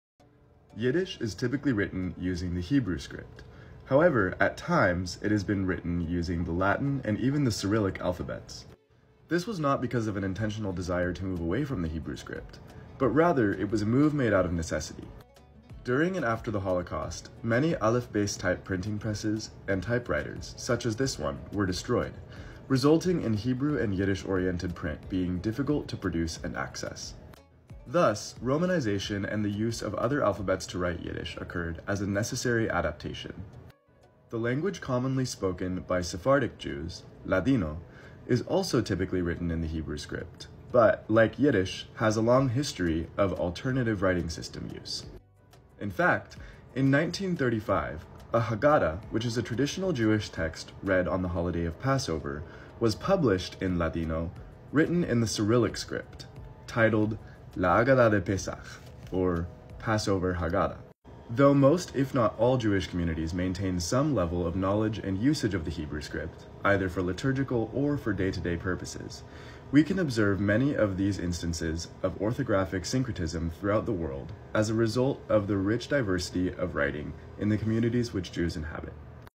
Writing Yiddish in Multiple Scripts
Here's Finn sharing how Yiddish used to be written not only in the Aleph-Beis script, but also in other scripts like Latin and Cyrillic, due to relocation, genocide, and other linguistic and social factors.